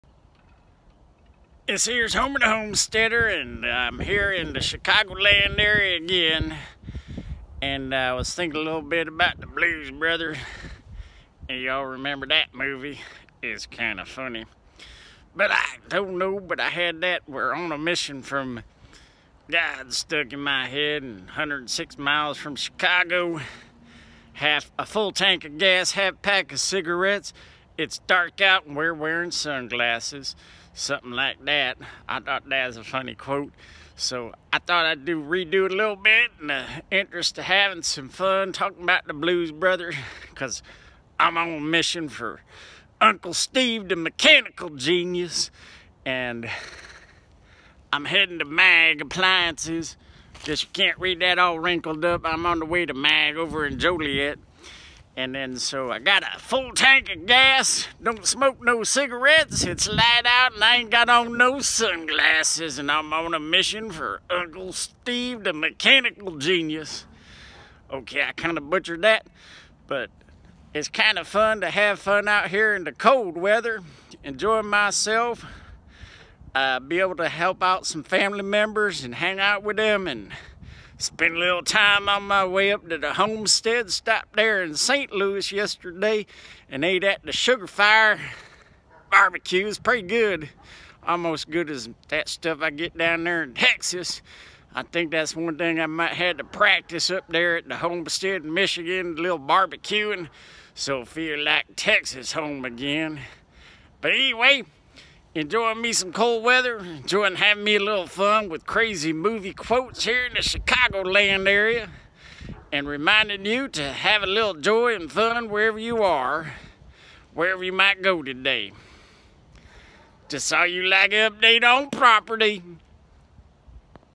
Thinking of The Blues Brothers, in Chicago!
Homer is up to good butchering movie quotes and having fun!